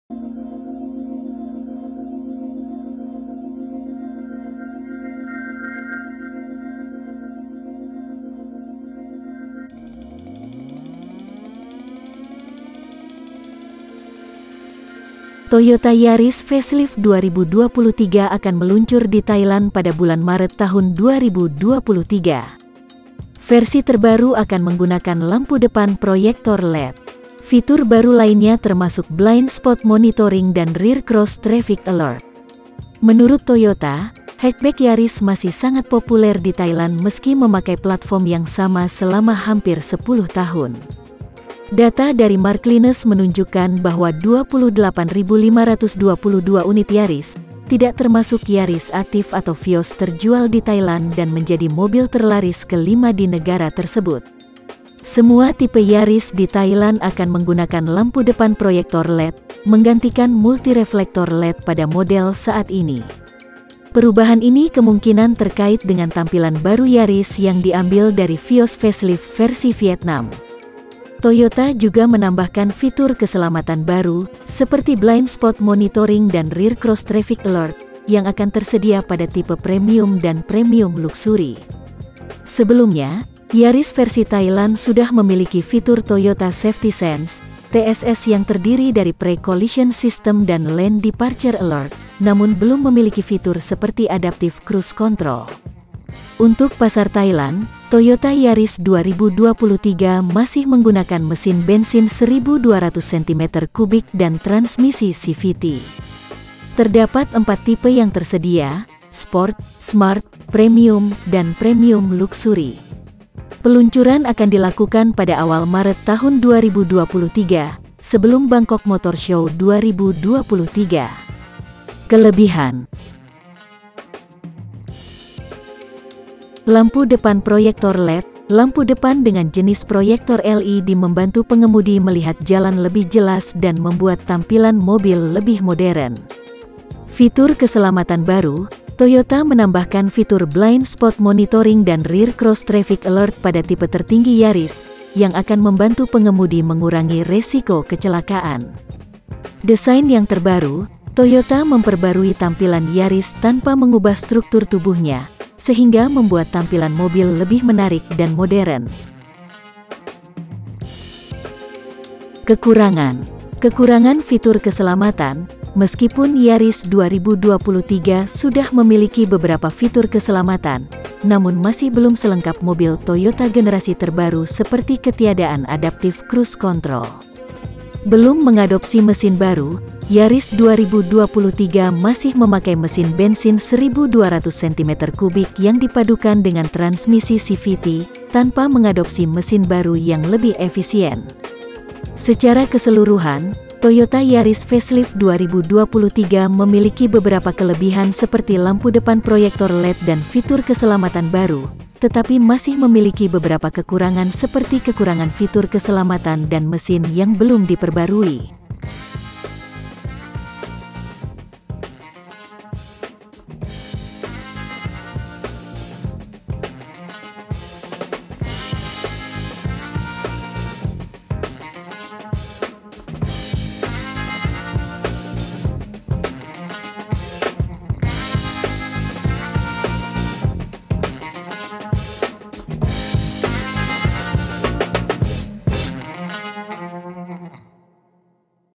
Toyota Yaris Facelift 2023 review Upgrade Lampu dan Fitur Keselamatan
Secara keseluruhan, Toyota Yaris facelift 2023 memiliki beberapa kelebihan seperti lampu depan proyektor LED dan fitur keselamatan baru, tetapi masih memiliki beberapa kekurangan seperti kekurangan fitur keselamatan dan mesin yang belum diperbarui.